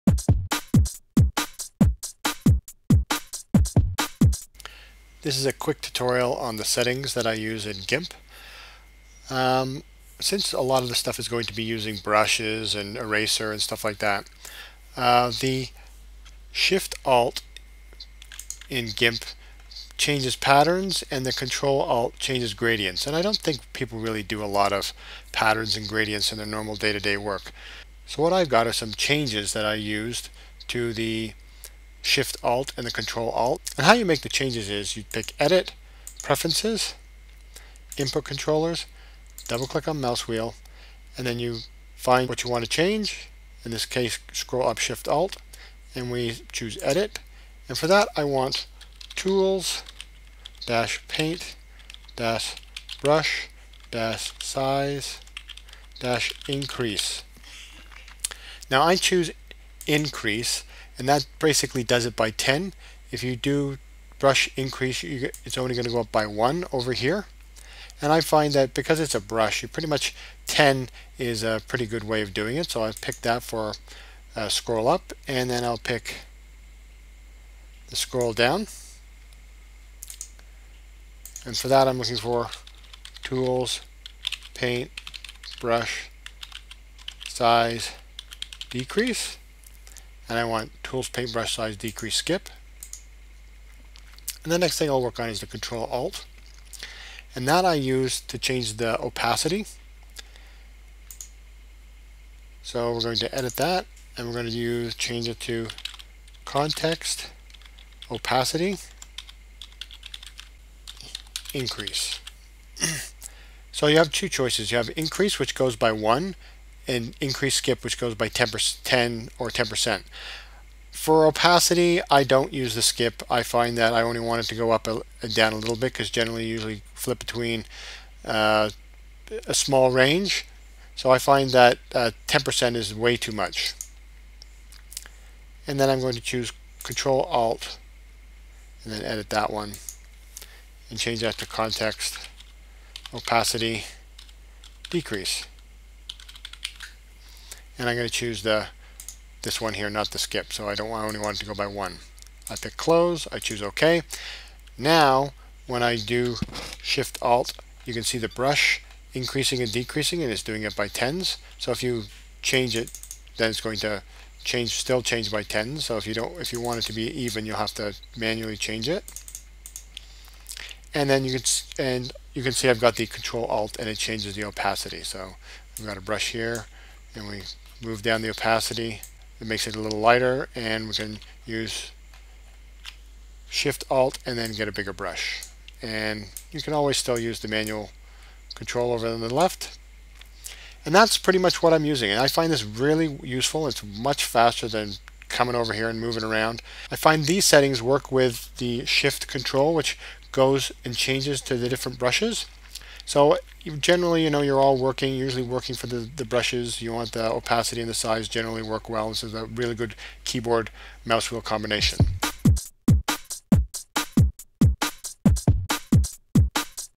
GIMP Tutorial: My Gimp Settings
These are the settings I use that allow me to adjust the brush size and bush opacity.
This works in version 2.6 & 2.8

Edit - Preferences - Input Controllers - Main Mouse Wheel

Brush Size
Scroll Up     (Shift + Alt) tools-paint-brush-size-increase-skip
Scroll Down (Shift + Alt) tools-paint-brush-size-decrease-skip

Opacity
Scroll Up      (Ctrl + Alt) context-opacity-increase
Scroll Down (Ctrl + Alt) context-opacity-decrease